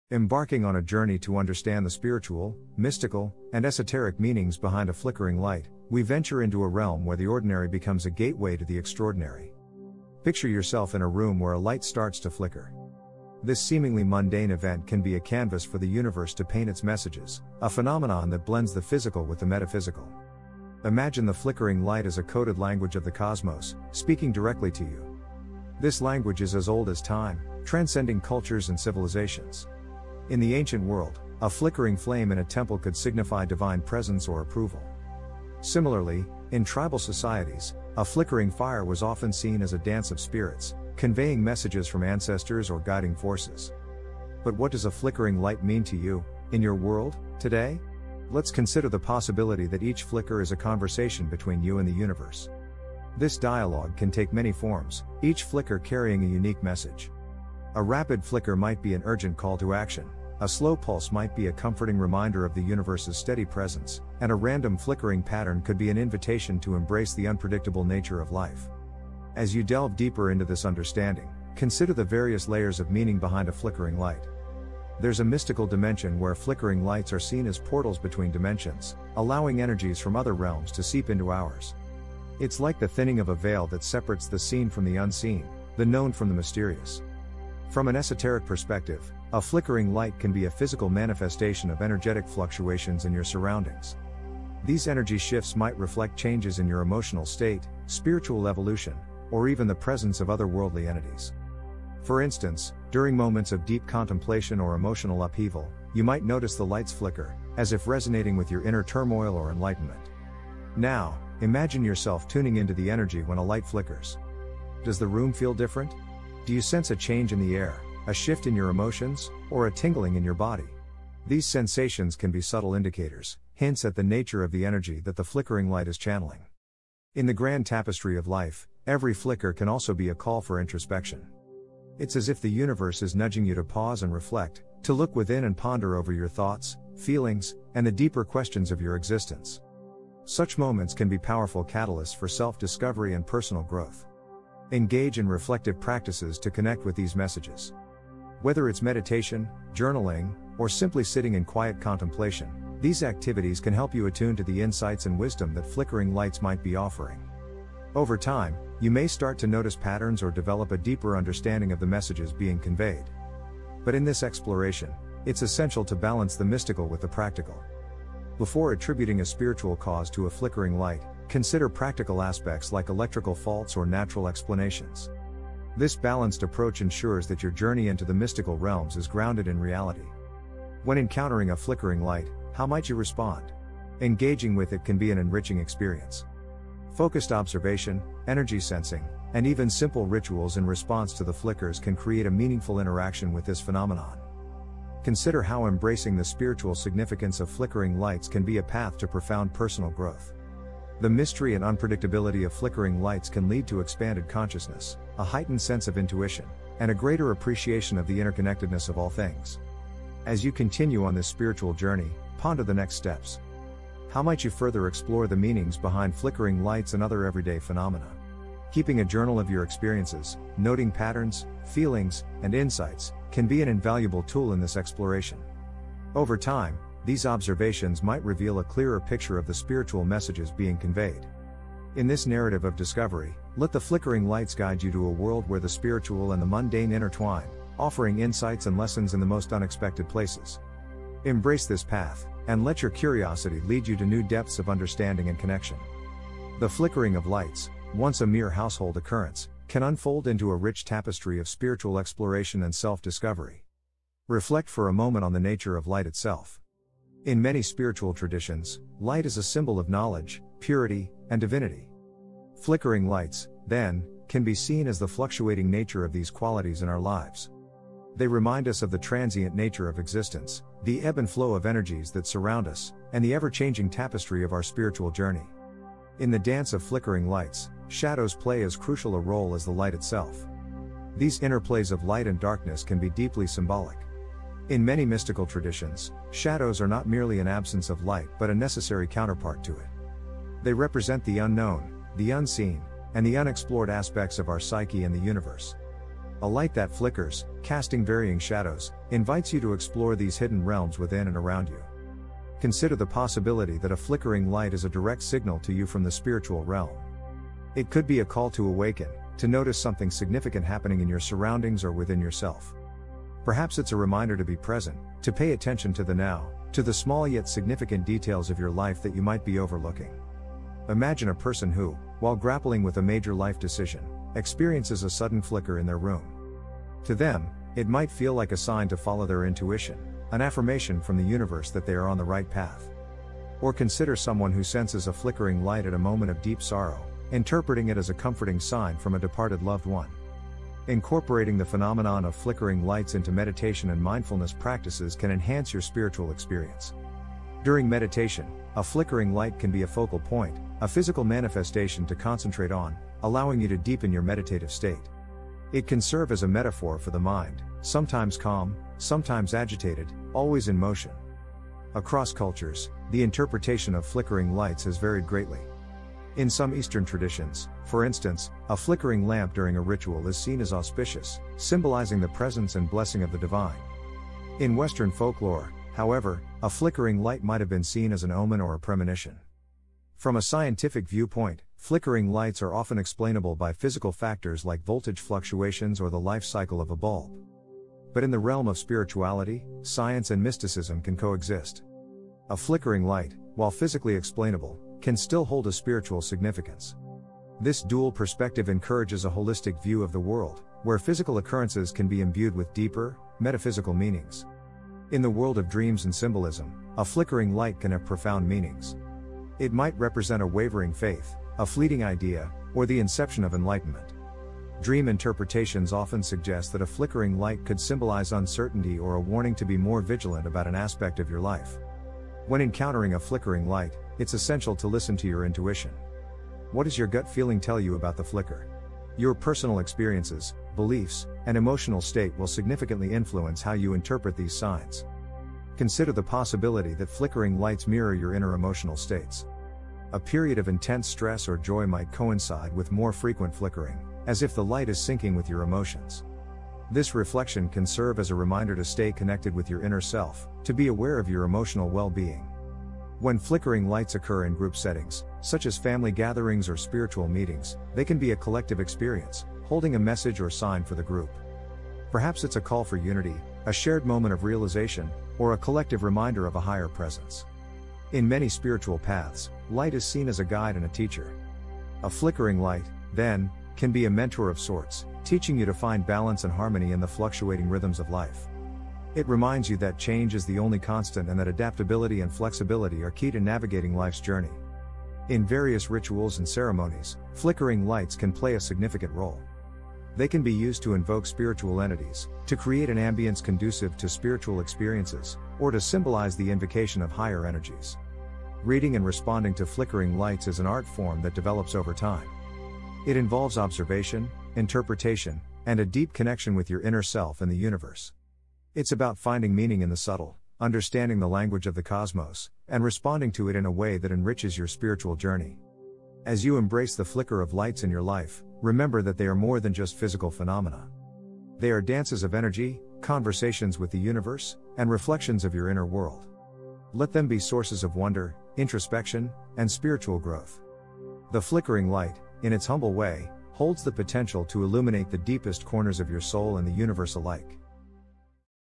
What Flickering Lights Are Urgently Telling You!
Dive into the enigmatic world of flickering lights in our latest video, exploring their mystical significance and cosmic connections. Uncover hidden messages and embrace spiritual growth through these captivating, mysterious phenomena. Get ready to be entranced by the universe's secrets!